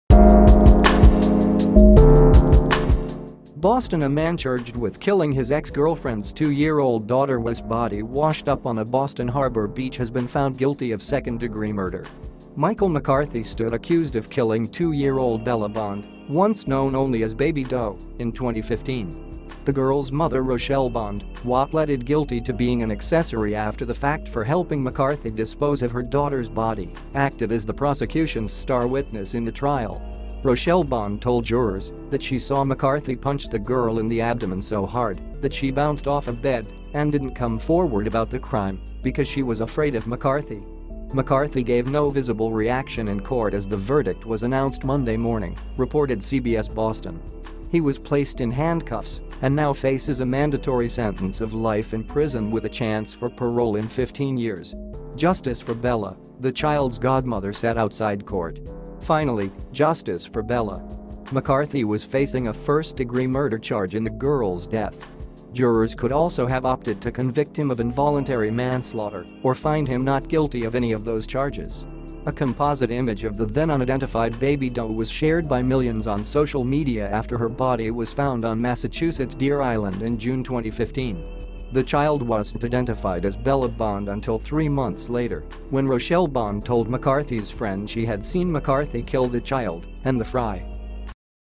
"Baby Doe" case: Michael McCarthy found guilty in death of ex's daughter, 2
BOSTON A mancharged with killing his ex-girlfriend's two-year-old daughterwhose body washed up on a Boston Harbor beach has been found guilty of second-degree murder.

Michael McCarthy stood accused of killing two-year-old Bella Bond, once known only as "Baby Doe," in 2015. The girl's mother Rachelle Bond, whopleaded guilty to being an accessoryafter the fact for helping McCarthy dispose of her daughter's body, acted as the prosecution's star witness in the trial. Rachelle Bond told jurors that she saw McCarthy punch the girl in the abdomen so hard that she bounced off a bed, and didn't come forward about the crime because she was afraid of McCarthy.

McCarthy gave no visible reaction in court as the verdict was announced Monday morning, reported CBS Boston. He was placed in handcuffs, and now faces a mandatory sentence of life in prison with a chance for parole in 15 years.

"Justice for Bella," the child's godmother said outside court. "Finally, justice for Bella."

McCarthy was facing a first-degree murder charge in the girl's death. Jurors could also have opted to convict him of involuntary manslaughter or find him not guilty of any of those charges.

A composite image of the then-unidentified "Baby Doe" was shared by millions on social media after her body was found on Massachusetts' Deer Island in June 2015. The child wasn't identified as Bella Bond until three months later, when Rachelle Bond told McCarthy's friend she had seen McCarthy kill the child, and the frie